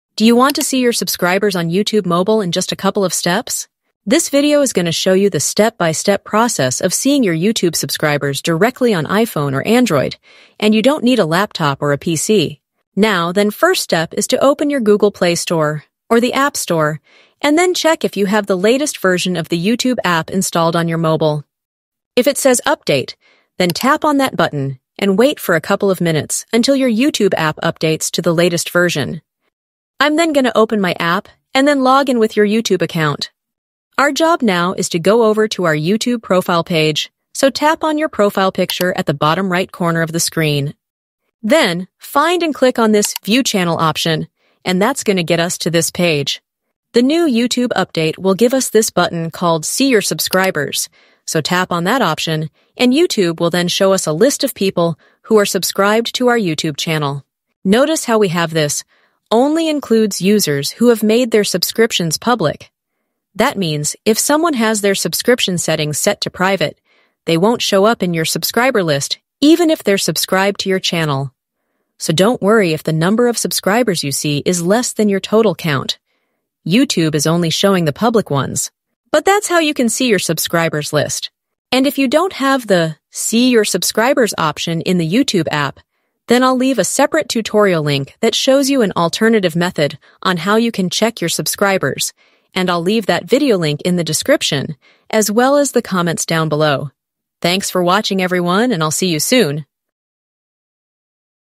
How To See Your Subscribers On YouTube Mobile I NEW METHOD
📱 In this quick and easy tutorial, I’ll walk you through the step-by-step process to view your YouTube subscribers directly on your iPhone or Android device — no laptop or PC needed!

✅ What you’ll learn in this video:

How to update the YouTube app to the latest version
How to access your channel page on mobile
⚠️ Keep in mind: Only users who have made their subscriptions public will be visible. So don’t worry if the number of subscribers shown is less than your total count!

(Also linked in the comments!)